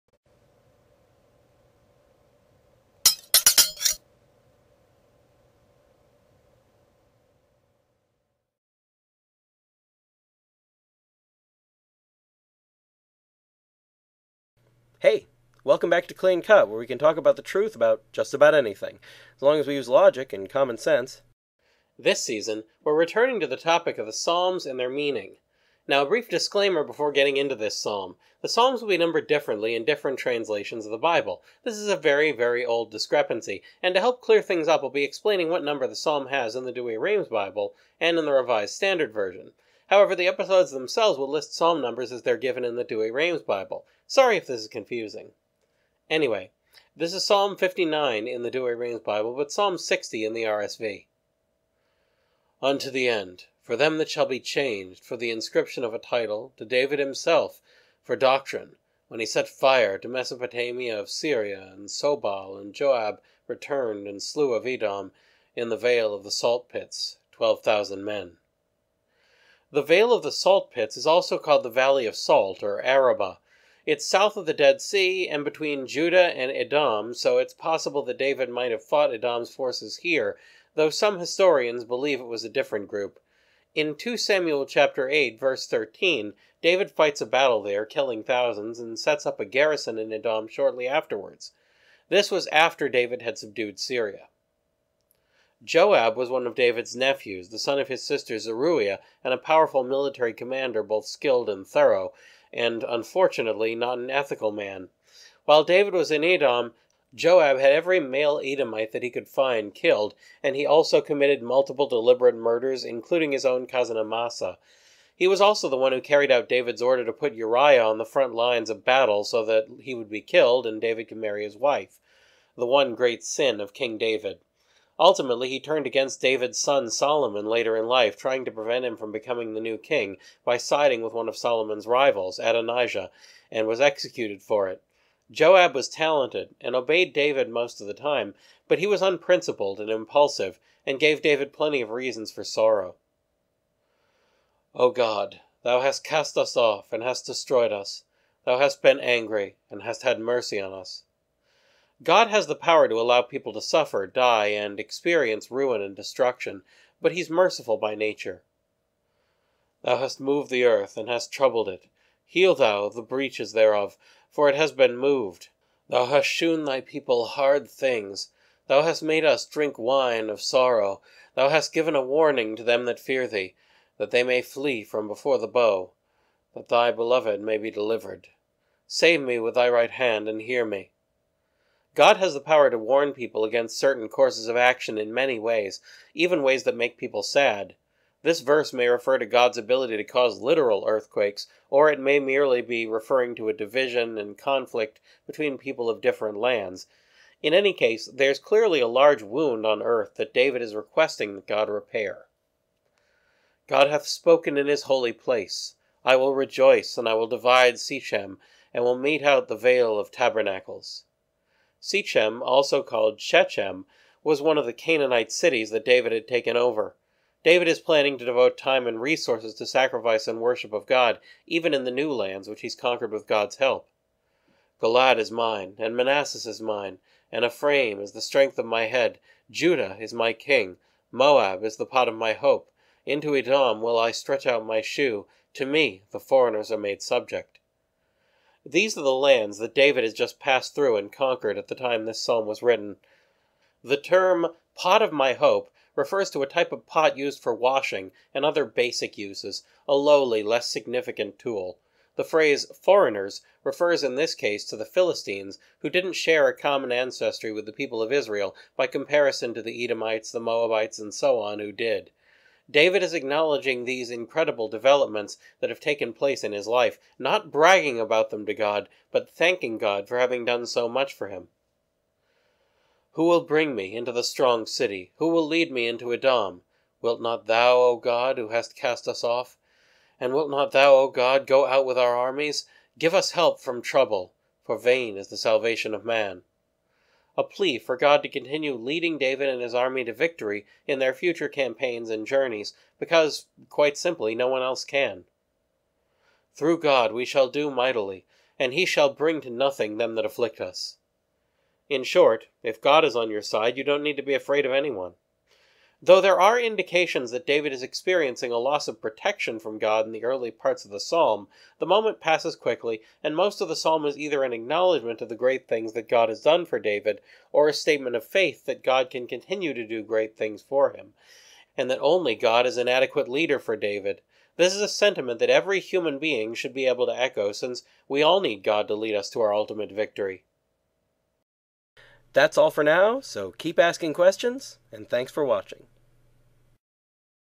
What's the Meaning of the Fifty-Nineth Psalm?
What was Arabah?  What kind of a person was Joab?  What was Shechem?
Due to YouTube censorship, the comment section is on BitChute now.